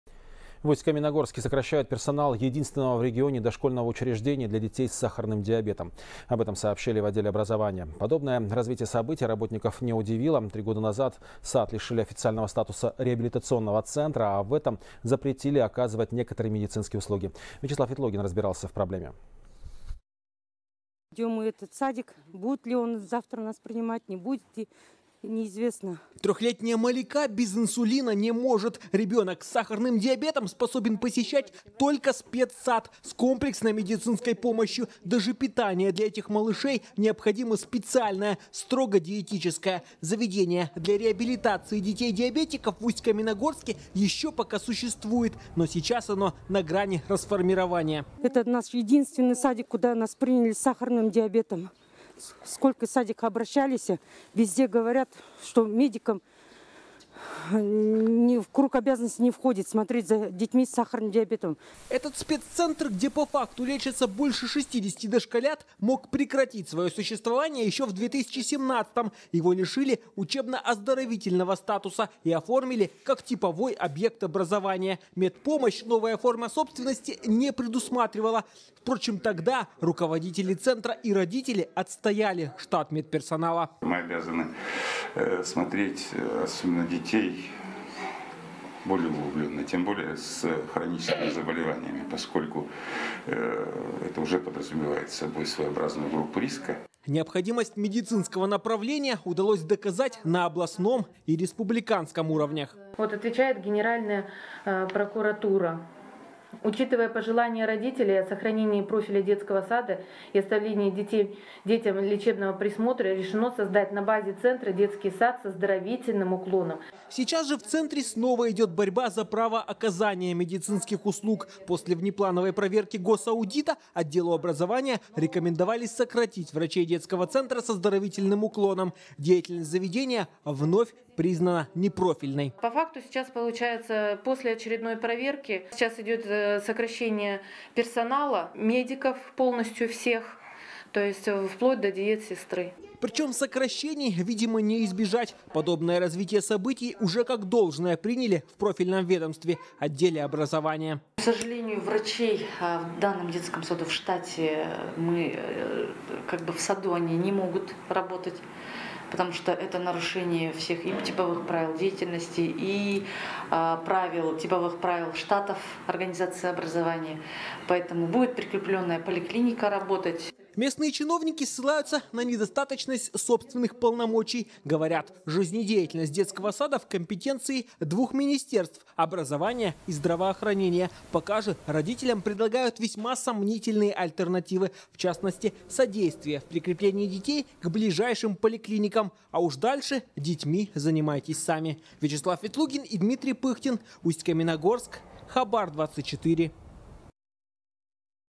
В Усть-Каменогорске сокращают персонал центра для детей-диабетиков
В Усть-Каменогорске сокращают персонал единственного в регионе дошкольного учреждения для детей с сахарным диабетом. Об этом сообщили в отделе образования. Подобное развитие событий работников не удивило. Три года назад сад лишили официального статуса реабилитационного центра, а в этом – запретили оказывать некоторые медицинские услуги.